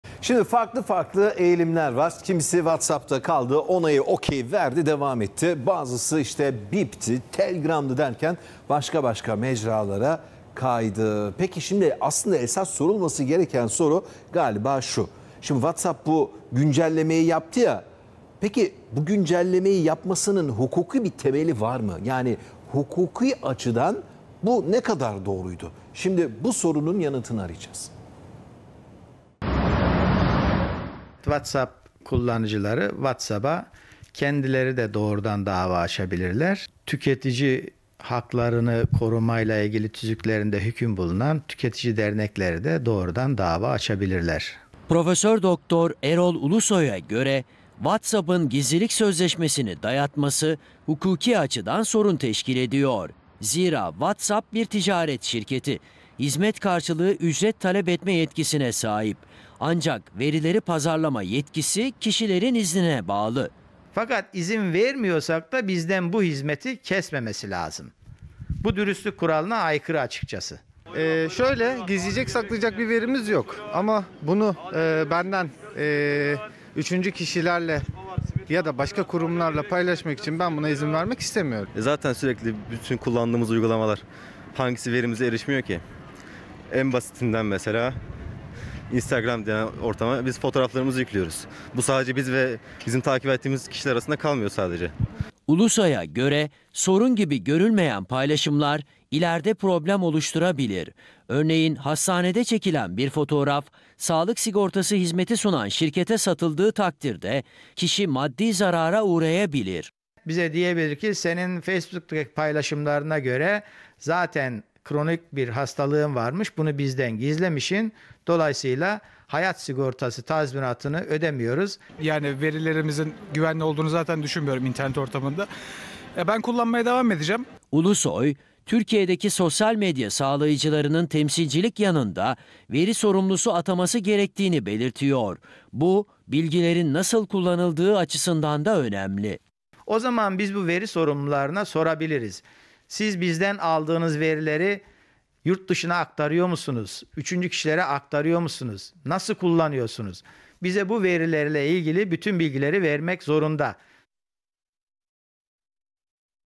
Kullanıcılar WhatsApp'a dava açabilir!
Akıllı telefonlarda en çok kullanılan uygulamalar arasındaki WhatsApp'a ilişkin gizlilik sözleşmesindeki değişiklikler tartışılıyor. WhatsApp'ın yaptığı güncellemenin hukuki temeli var mı?